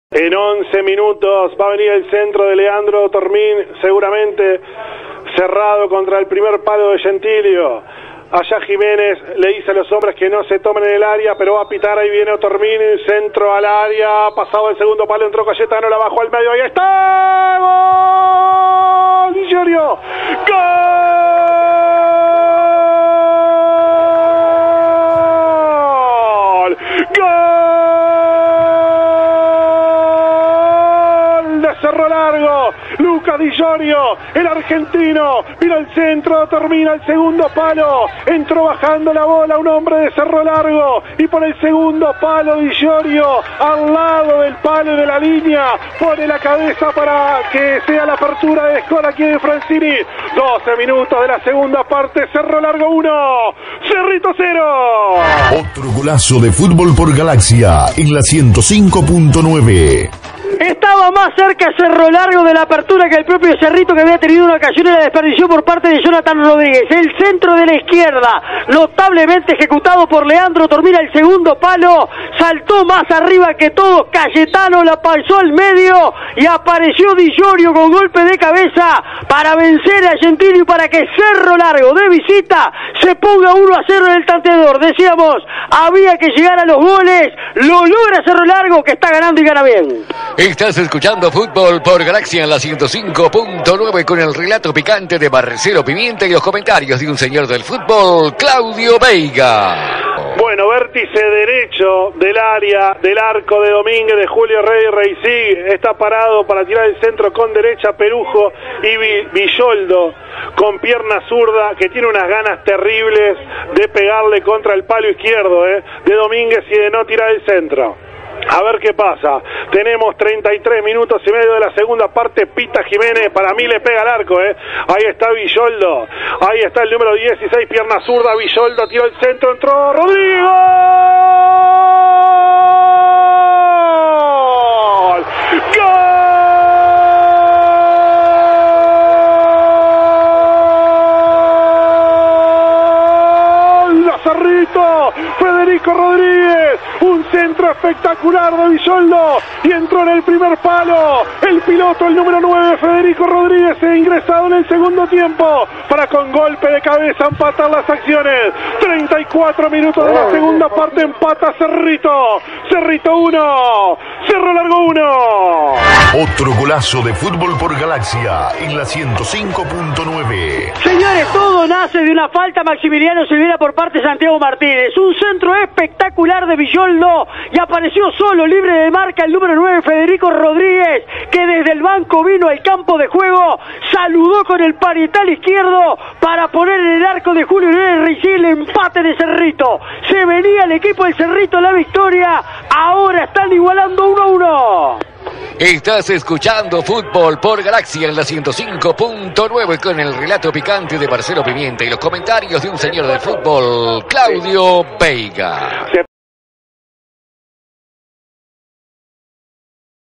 Cerrito 1 Cerro Largo FC 1 - Clausura 2021 (por Marcelo Pimienta)
Los goles del partido, narrados por Marcelo Pimienta y comentados por Claudio Veiga para Emisora Galaxia.